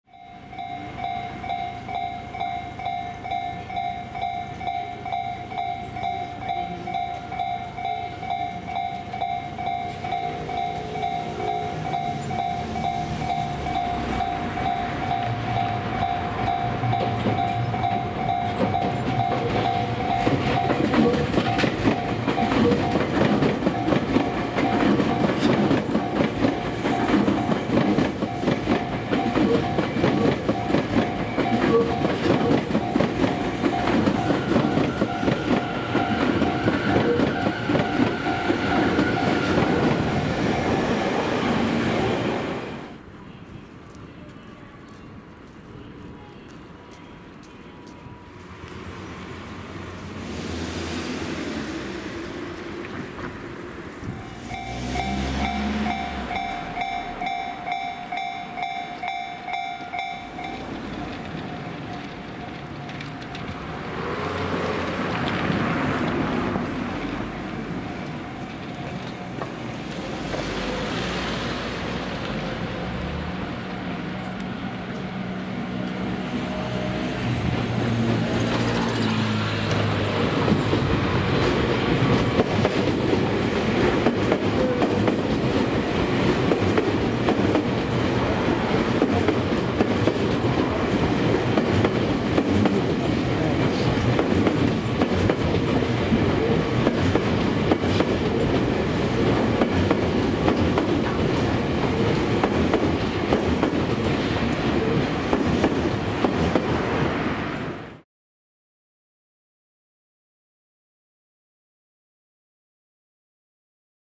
成増駅引き上げ線発着 東武東上線50000系51001F/Tobu Railway 50000 Series at Tojo Line Narimasu Station/2017.12.22
車両: 東武鉄道50000系51001F
撮影場所: 東上線成増駅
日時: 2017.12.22

Train: Tobu Railway 50000 Series
Location: Tojo Line Narimasu Station (Tokyo, Japan)
Date: December 22, 2017